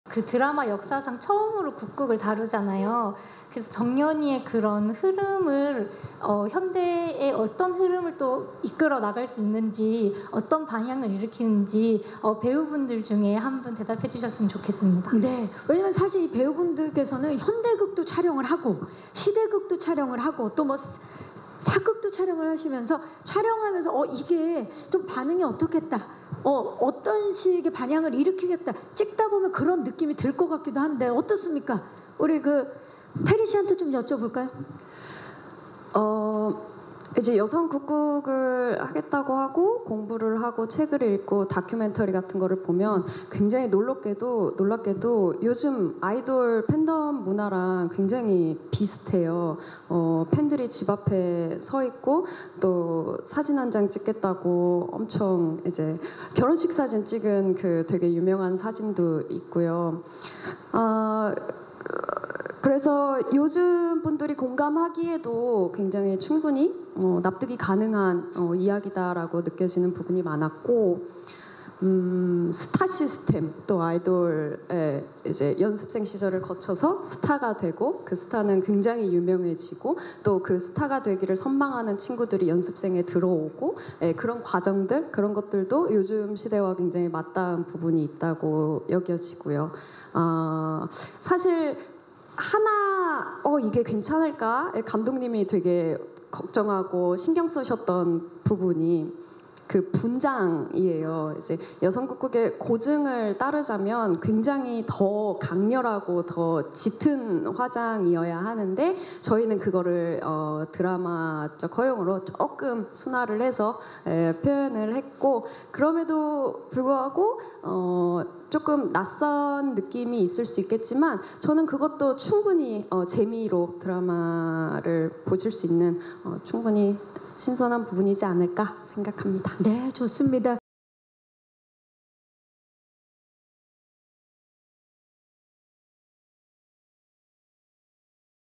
[4K] '정년이' 김태리, 아이돌 팬덤 문화와 비슷한 국극 (정년이 제작발표회) KimTaeri 2024.10.10 Newsen
[뉴스엔 최재현 기자]

10월 10일 오후 서울 영등포구 국제금융로 콘래드 서울 호텔에서 tvN ‘정년이’ 제작발표회가 열렸다.

현장에는 배우 김태리, 신예은, 라미란, 정은채, 김윤혜, 정지인 감독이 자리해 작품과 관련한 이야기를 나누었다.

한편, ‘정년이’는 1950년대 한국전쟁 후를 배경으로, 최고의 국극 배우에 도전하는 '타고난 소리 천재' 정년이를 둘러싼 경쟁과 연대, 그리고 찬란한 성장기를 그린 작품으로, 10월 12일 첫 방송된다.

·'뉴스엔' 채널 구독하세요!